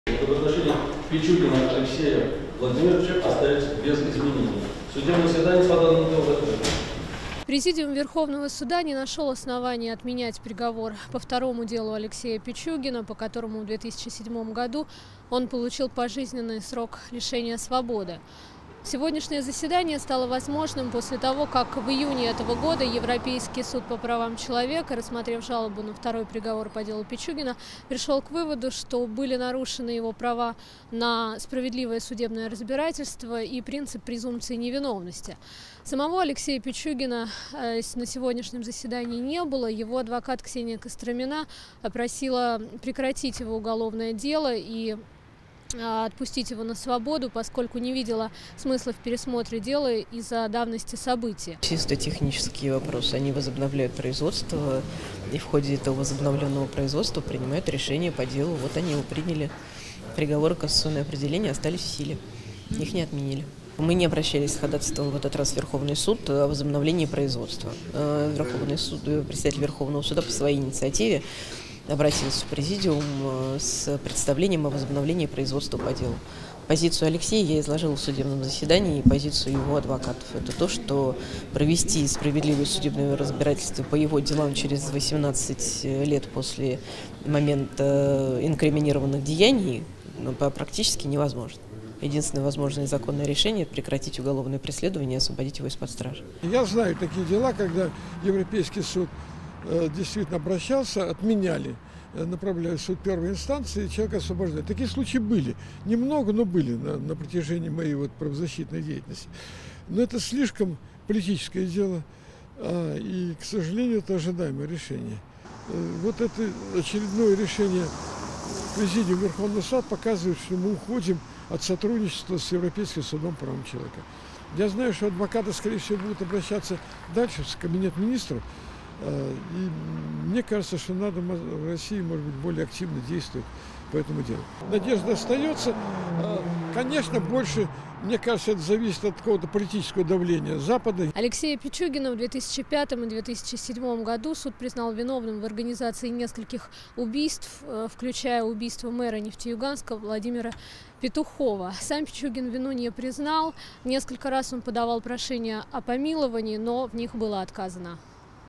Верховный суд оставил в силе приговор Алексею Пичугину
Верховный суд России в среду подтвердил приговор бывшему руководителю службы внутренней экономической безопасности ЮКОСа Алексею Пичугину. Пересмотр дела был назначен 24 октября в связи с постановлением, которое вынес Европейский суд по правам человека (ЕСПЧ). Пичугин жаловался на несоблюдение принципа презумпции невиновности и нарушения во время расследования. Летом этого года суд в Страсбурге присудил ему компенсацию в 15 тысяч евро, а защита осужденного потребовала рассмотреть его дело повторно. Алексей Пичугин с 2007 года отбывает пожизненное наказание в колонии "Черный дельфин" в Оренбургской области по обвинению в организации нескольких убийств. Бывший сотрудник ЮКОСа неоднократно заявлял о своей невиновности, а также о том, что следствие пыталось склонить его к ложным показаниям против руководства нефтяной компании. Суд не удовлетворил ни одно из прошений Пичугина о помиловании. Правозащитный центр "Мемориал" признал его политическим заключенным.